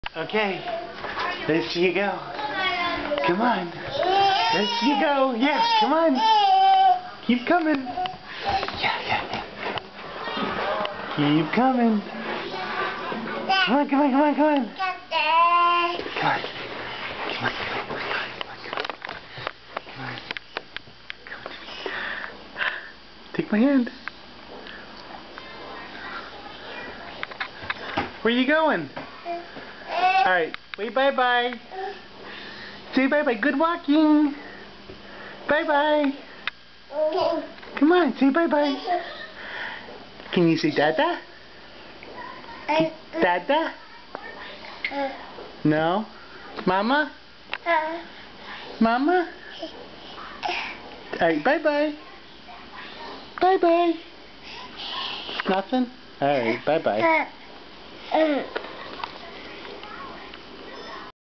Nora is moving now
Nora walk around the house